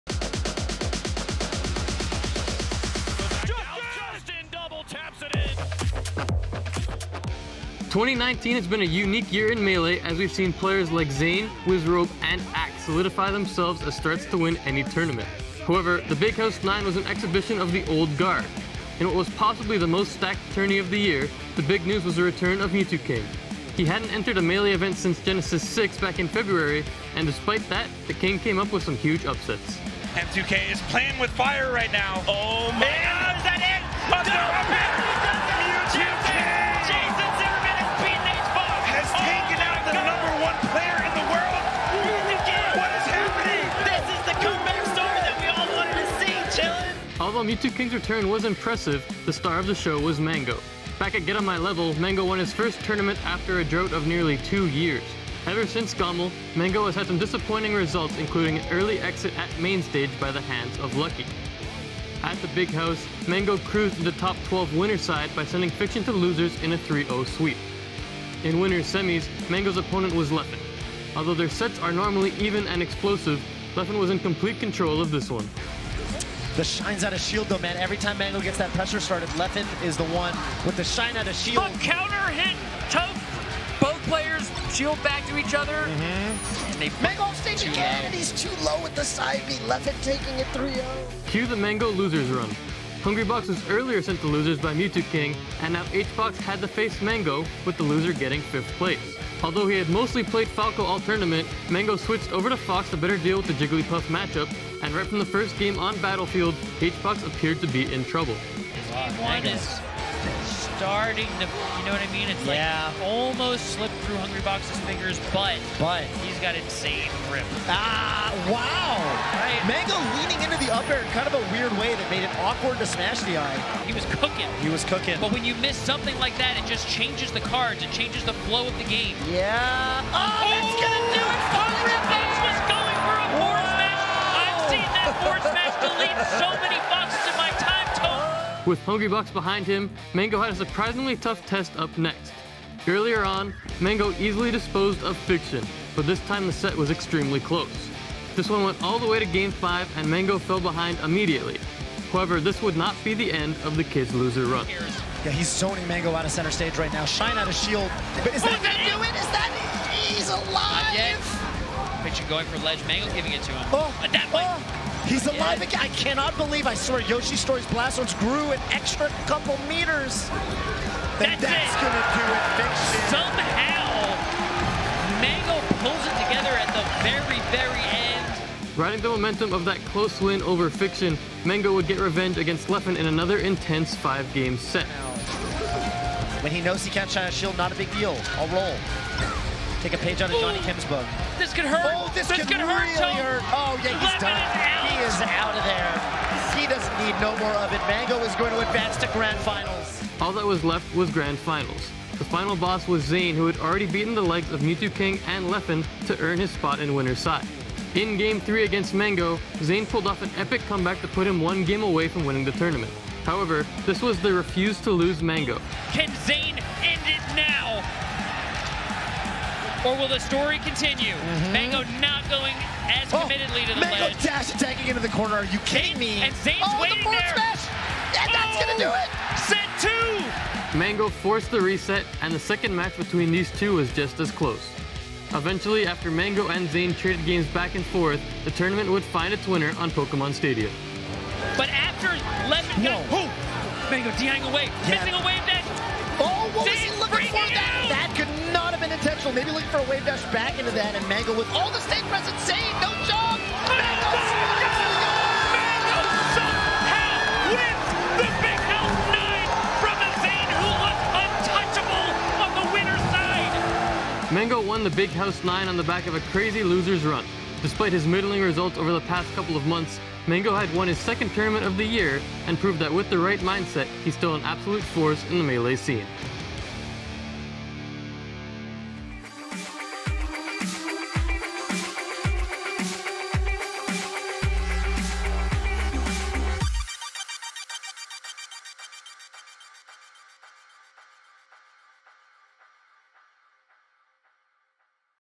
Mang0's Wild Losers Run at The Big House 9 | The Flex
The Kid still has it. After getting dropped to losers side by Leffen, Mang0 went on an epic run to win The Big House 9.